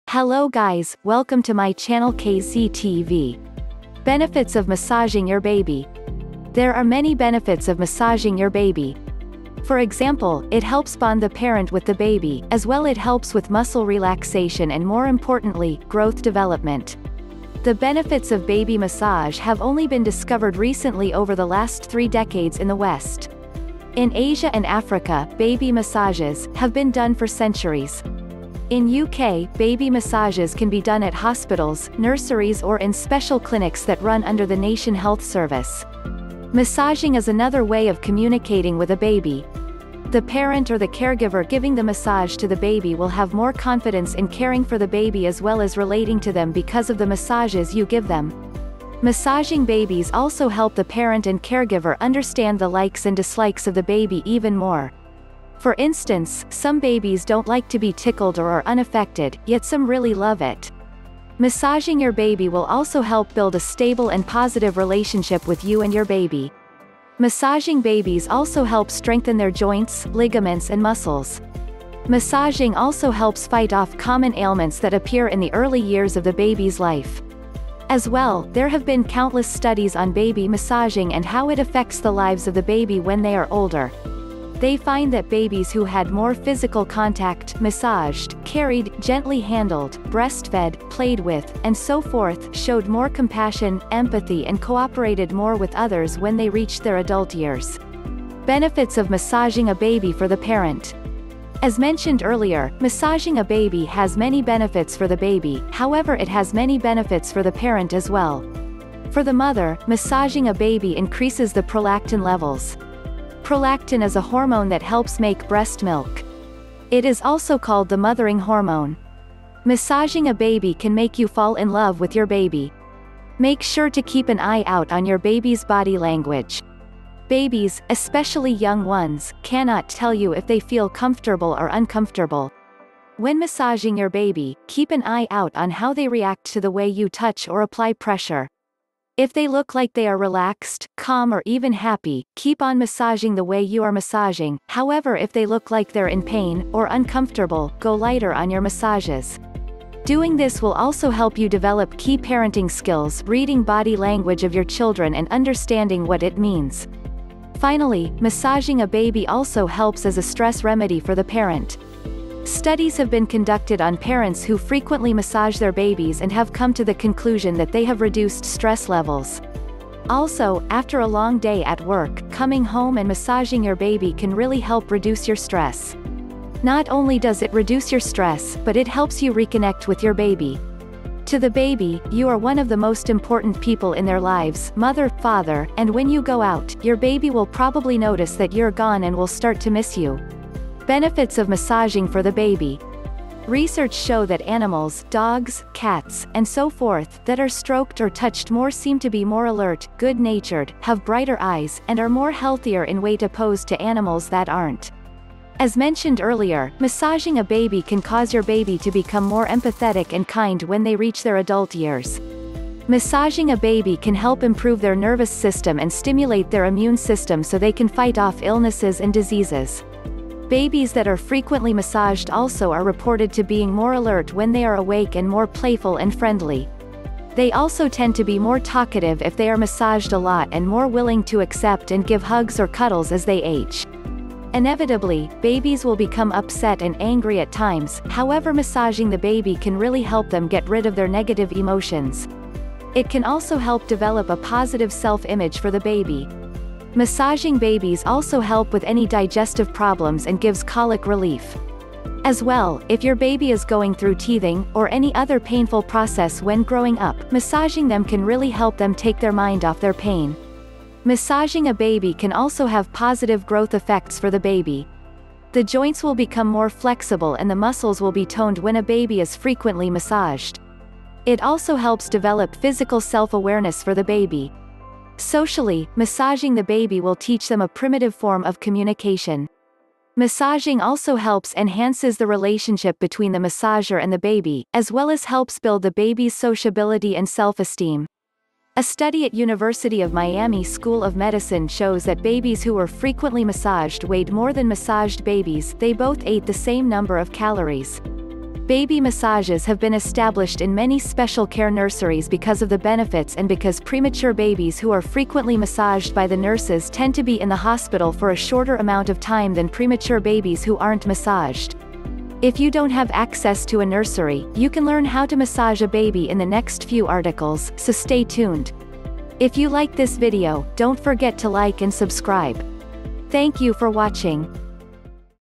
BENEFITS OF MASSAGING YOUR BABY
There are many benefits of massaging your baby. For example, it helps bond the parent with the baby; as well it helps with muscle relaxation and more importantly, growth development. The benefits of baby massage have only been discovered recently over the last 3 decades in the west. In Asia and Africa, baby massages, have been done for centuries. In UK, baby massages can be done at hospitals, nurseries or in special clinics that run under the nation health service.